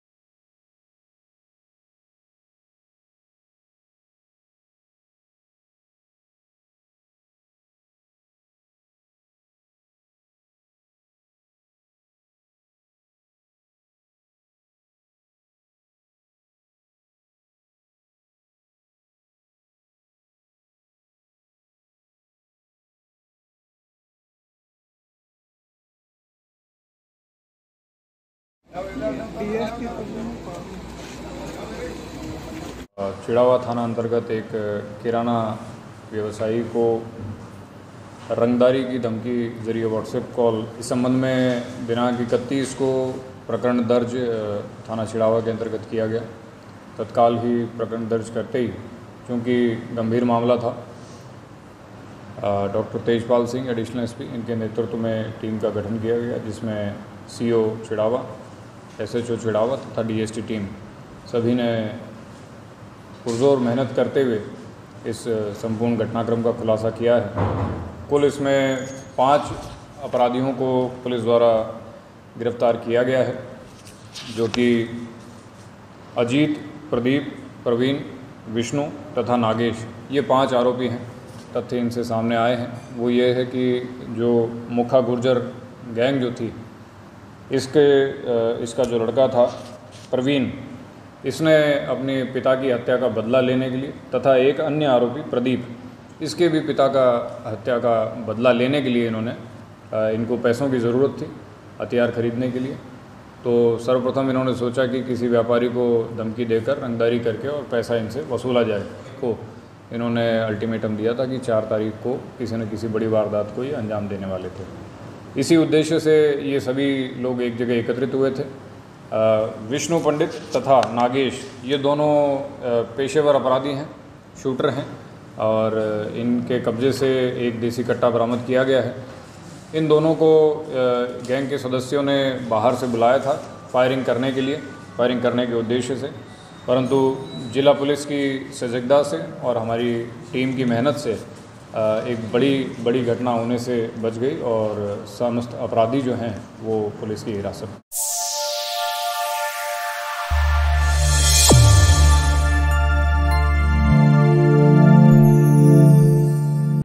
5 बदमाशों को पुलिस ने किया गिरफ्तार, ASP के निर्देशन में संयुक्त कार्रवाई | Jhunjhunu News
About Video:
5 बदमाशों को पुलिस ने किया गिरफ्तार, ASP के निर्देशन में संयुक्त कार्रवाई | Jhunjhunu News|  2 June 2023